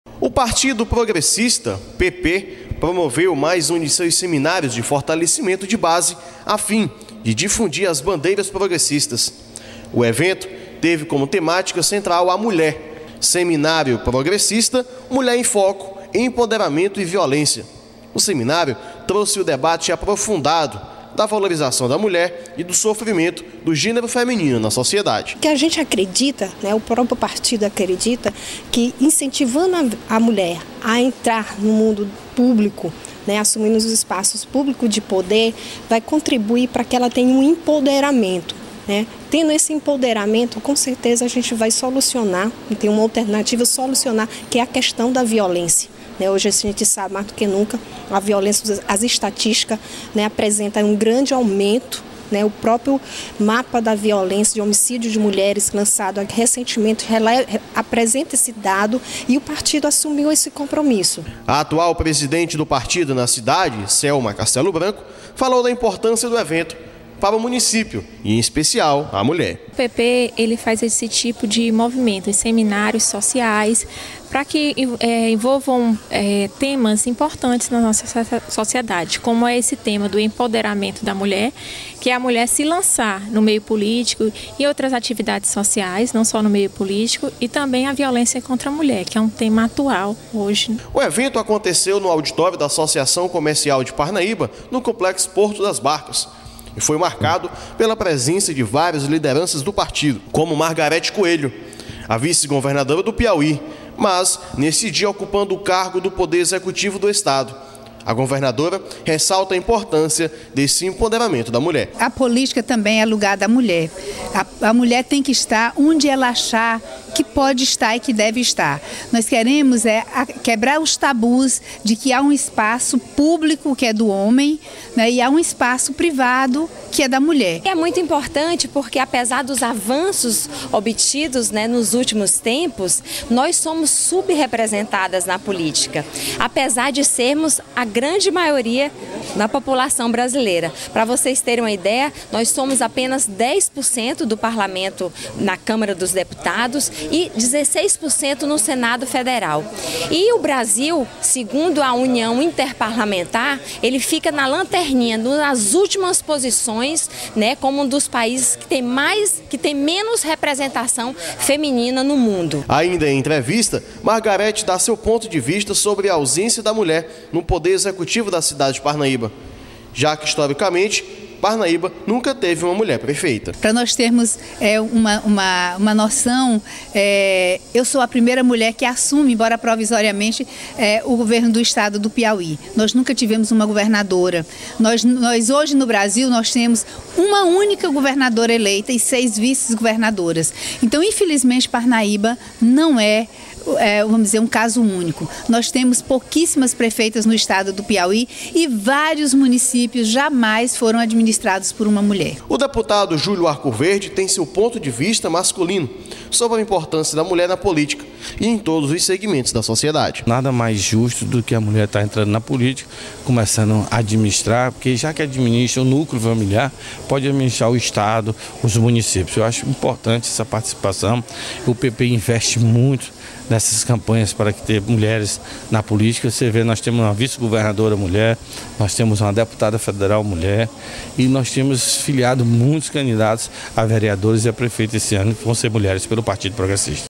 Mulher em foco
Na última sexta-feira a vice-governadora do estado, Margarete Coelho  cumpriu agenda aqui em Parnaíba e em Luís Correia.Uma das atividades foi o seminário mulher em foco que abordou o tema "Empoderamento e Violência contra a Mulher".